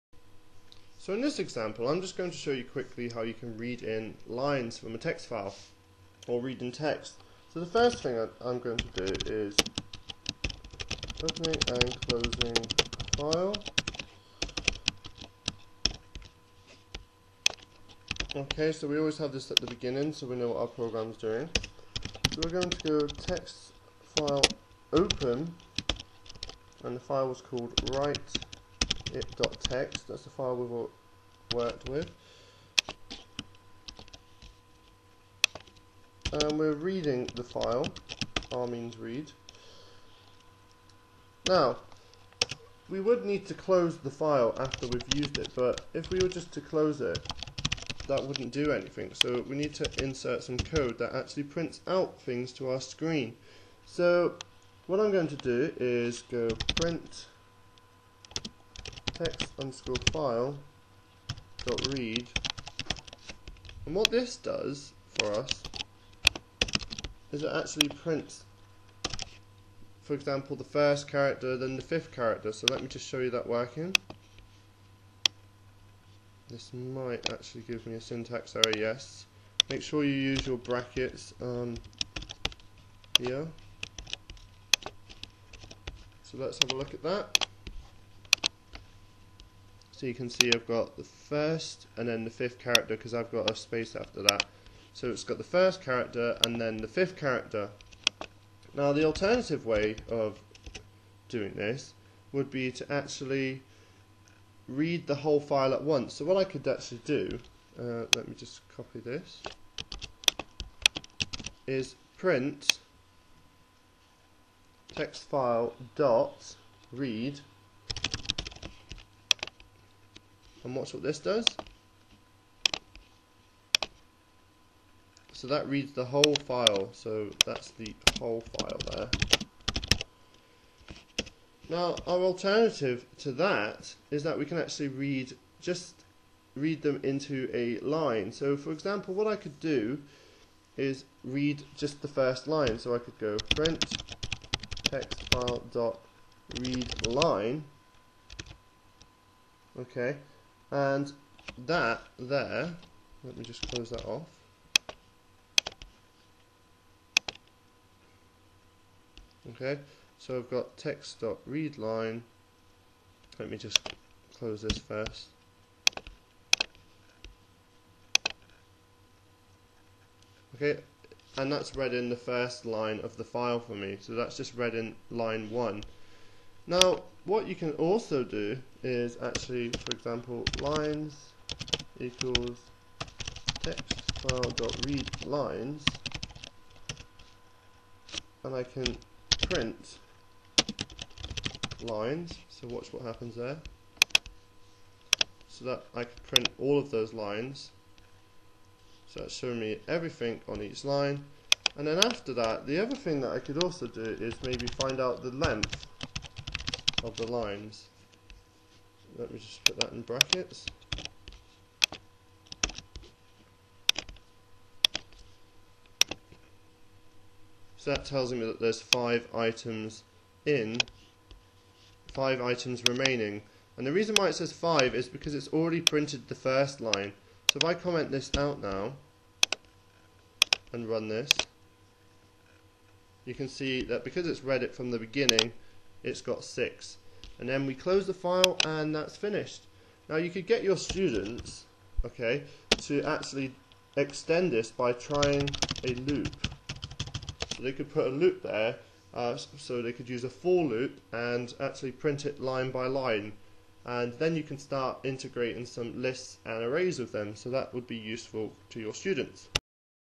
Reading text from a file - Python 3.3
A short lesson which shows you how to read back the information from the text file you have created. It is a follow on form the lesson on writing to a text file. It should help out those people new to computer science and it is a nice stepping stone. Computer Science GCSE is the way forward!